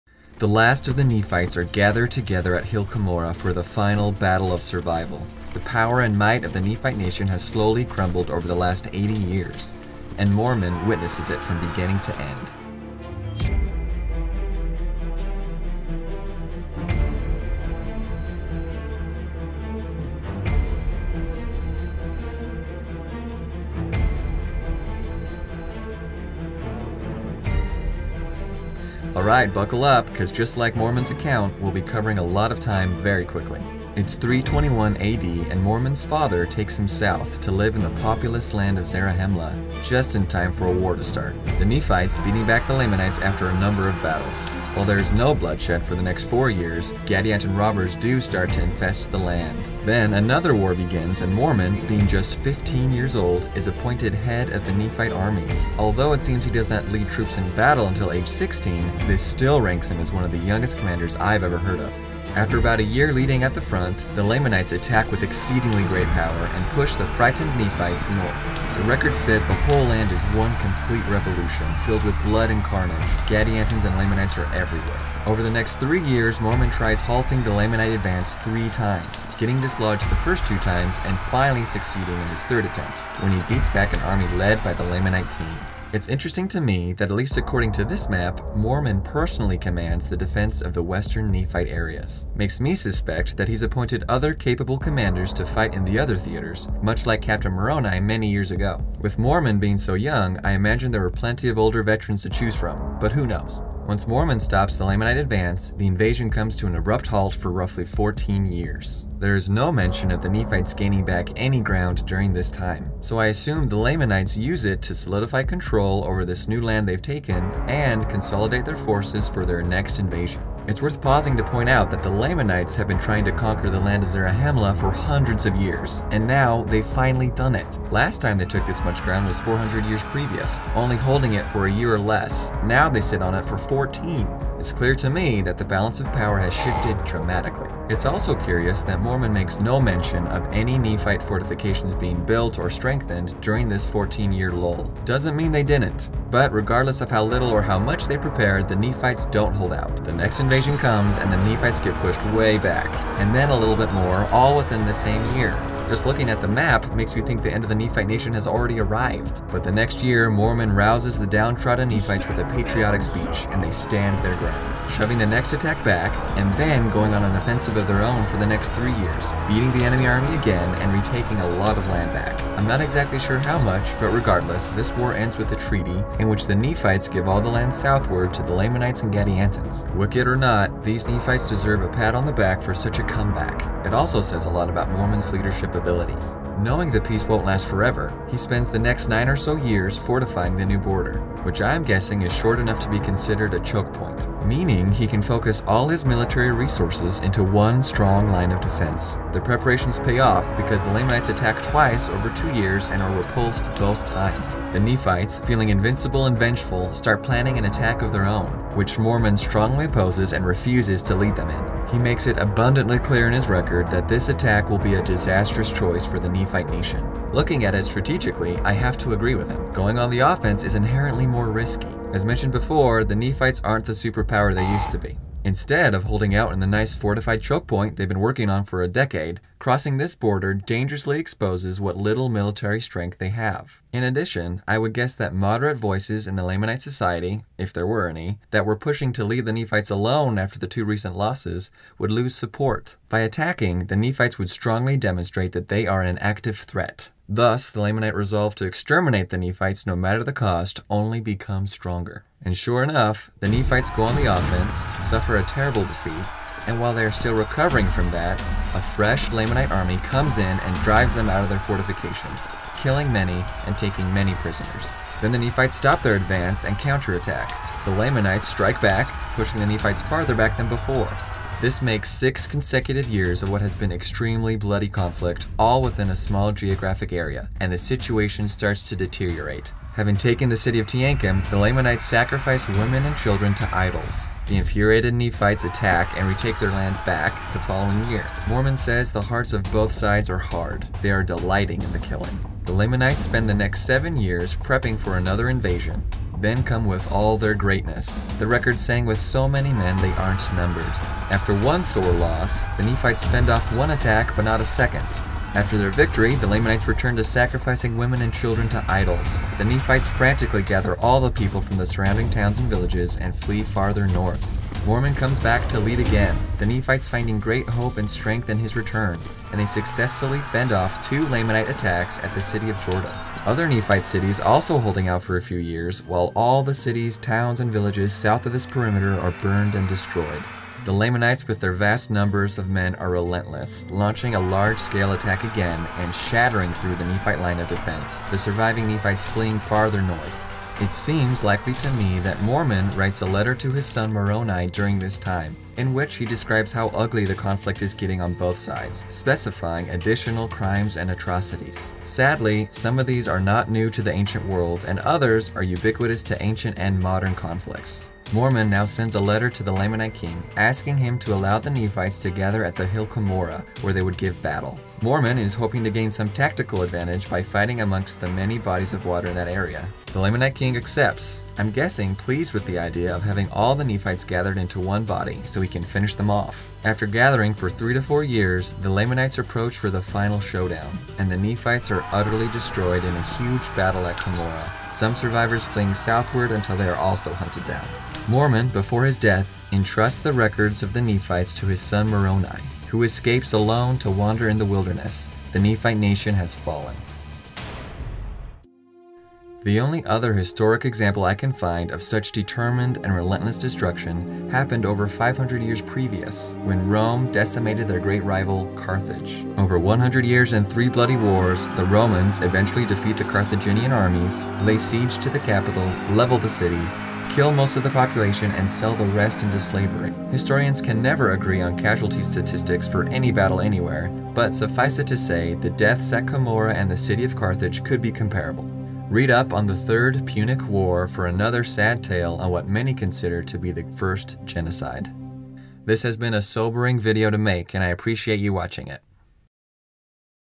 The Fall of the Nephites | Mormon 1-8 | Book of Mormon Battles
Book of Mormon wars explained and analyzed through the lens of a military history nerd  - The Fall of the Nephites (Mormon 1-8 ... page 469). Enjoy!

Mormon doesn't provide many details about the conflicts I summarize in this video, as such I've had to input a lot of personal estimates. I try to acknowledge these estimates by using dotted lines and question marks for uncertain borders/battle locations. I welcome any questions or critiques of these estimates.

Map adapted from this artistic rendition (does not represent a specific geographic region)

Why I went with this map -"The Church does not take a position on the specific geographic locations of Book of Mormon events in the ancient Americas... Individuals may have their own opinions regarding Book of Mormon geography ... However, [the Church urges] members not to advocate those personal theories in any setting or manner that would imply either prophetic or Church support for those theories. All parties should strive to avoid contention on these matters."

Click here for more on young Mormon and why he may have been chosen as the Nephite military leader.

Summary of young military leaders below:

-Mormon: 16
-Americas. Lamanite Invasion (under King Aaron). 326 AD
-Put in overall command at 15, led troops at 16 (Mormon 1:15-2:2)

-Alexander the Great: 16
-Macedon. The Maedoi revolt. 340 BC
-Alexander puts down another revolt at age 17. Both were done while his father was away on campaign. He doesn’t technically take overall command until his kingship at 20 years of age

-Muhammad bin Qasim: 17
-Saudi Arabia 708-711 AD, conquest of Singh and Punjab (Pakistan)
-Some estimates put him as young as 15

-Joan of Arc (spelled wrong in video): 17
-France. Siege of Orleans 1429

-Wladyslaw III: 17
-Poland, lead armies against Elizabeth of Luxembourg. 1442
-His most famous battle came at 19 when he was beheaded at the Battle of Varna 1444

-Scipio Africanus: 18
-Italy. Battle of Ticinus 218 BC
-Led a cavalry charge to save his father at Ticinus. Scipio later appointed proconsul at age 25

-Charles XII: 18
-Sweden. Great Northern War, 1700

For the list of young military leaders above I referenced superficial articles like these and dug deeper on each individual to get more of the background.

Speculations on Siege of Carthage death statistics:
-up to 650K killed and 50K enslaved

Consider what this paper published by Yale summarizes (The First Genocide: Carthage, 146 BC - by Ben Kiernan)

"Strabo put Carthage's population c.149 BC at 700,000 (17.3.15). B.H. Warmington considers this impossible and suggests 200,000, though 'in the early third century . . . it would not be surprising if it not approach 400,000' (Carthage, London, 1980, 124-7). Appian reported the population rose 'greatly' after 201 BC (Roman History 8.10.69), as archaeology has 'fully' confirmed' (Ursula Vogel-Weidemann, 'Carthago Delenda Est: Aitia and Prophasis',Acta Classica, XXXII, 1989,79-95, at 86-7). Huss adds that during the siege, 'large sections of the rural population took refuge within the city walls' (Geschicte,452)"